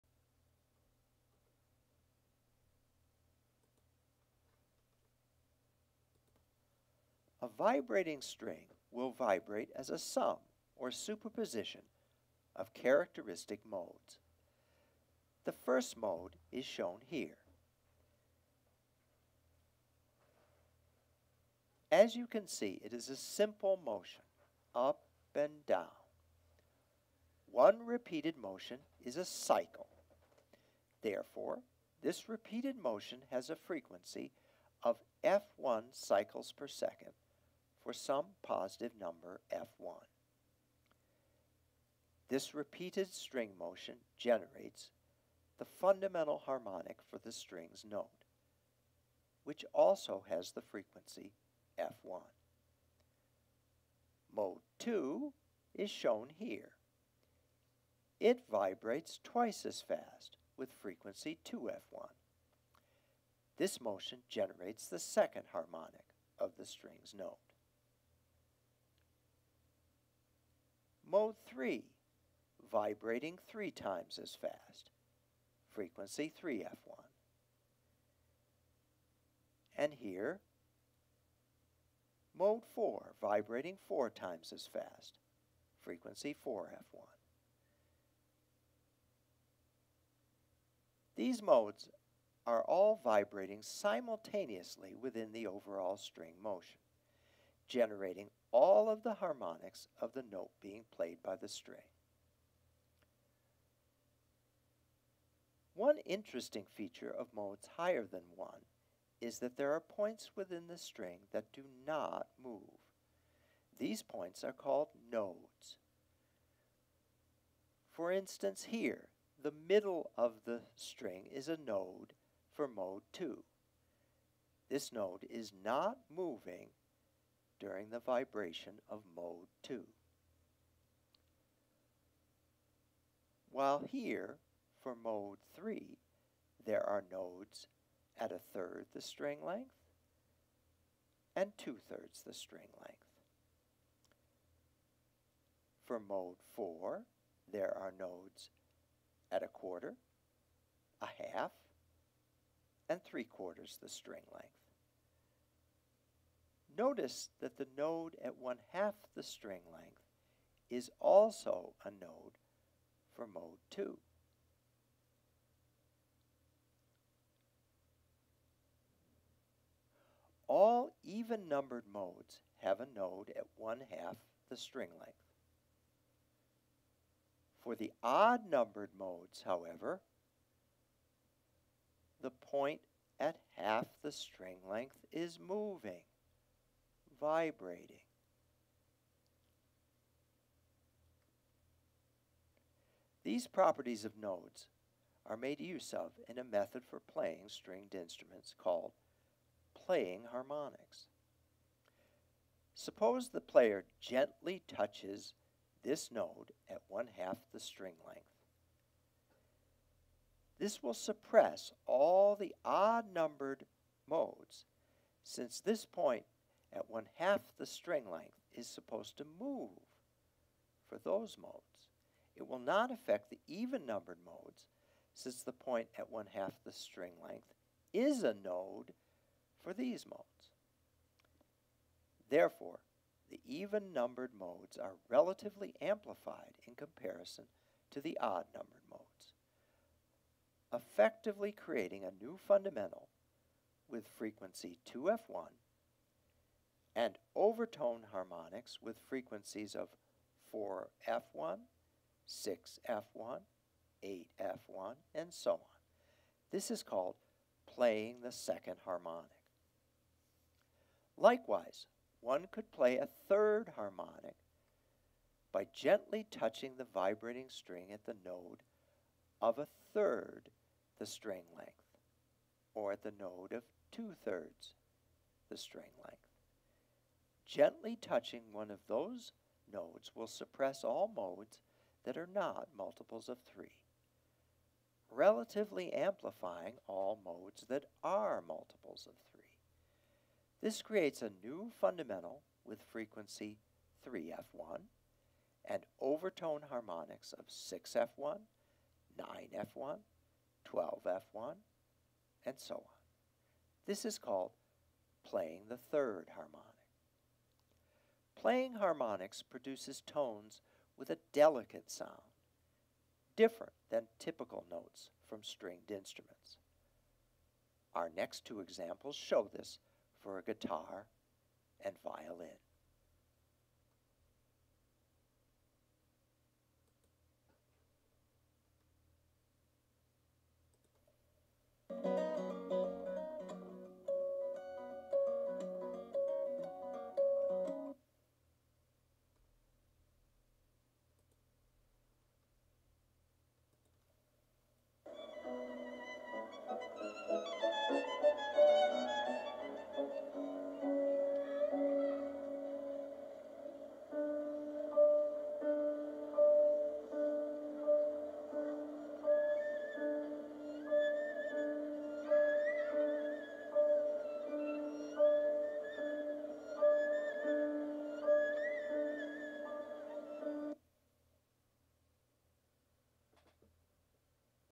Vibrating String Harmonics (Slideshow)
A brief summary of the theory of characteristic modes for string vibration.  Musical examples illustrate how the theory of nodes for these modes explains the method of playing harmonics.